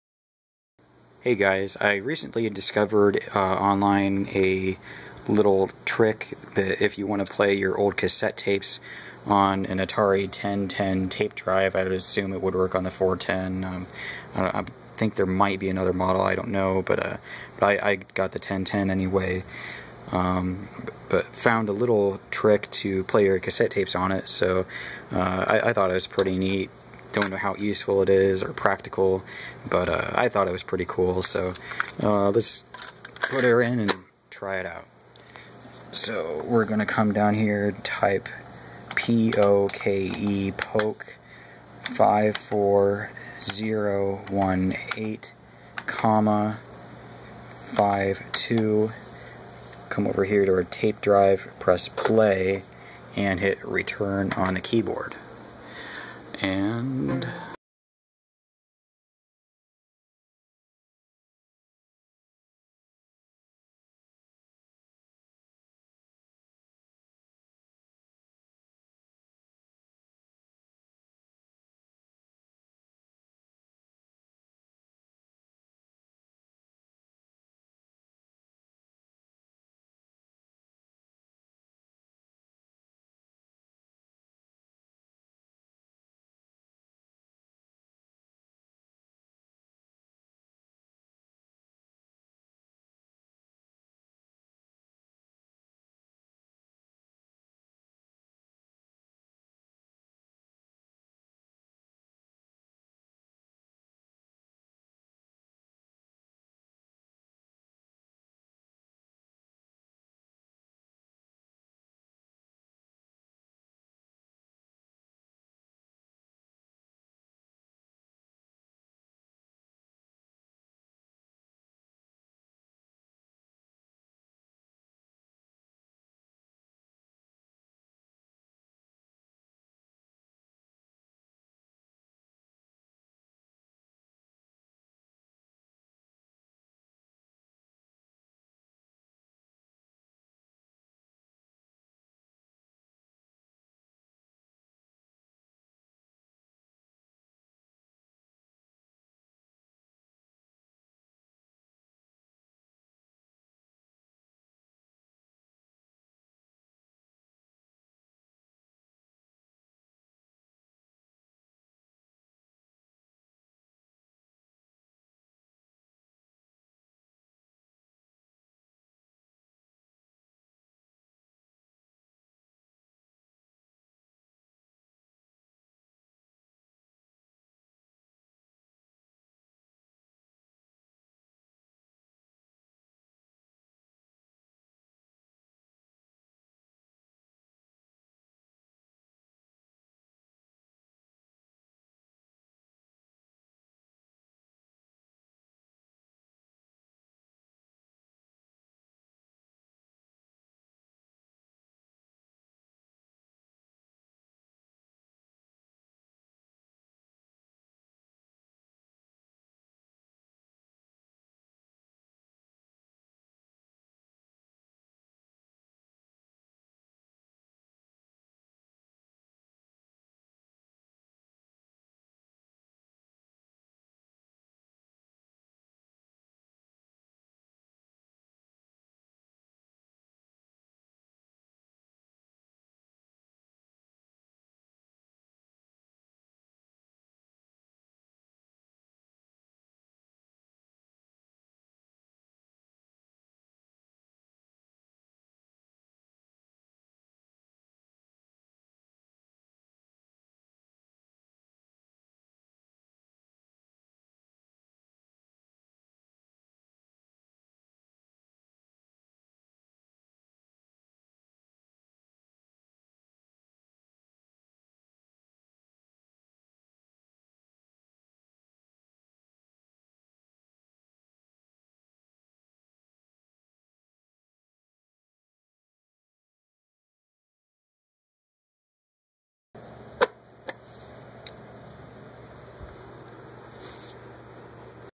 How to play an audio cassette on an Atari 8-bit computer.
I found a neat little trick on Atariage.com on how to "POKE" / get the motor to run on the Atari tape drive. This allows for continuous playback of whatever tape is currently inside the drive. You can use the other functions of the drive (STOP, REWIND, ADVANCE) without having to retype the command.

Command: POKE 54018,52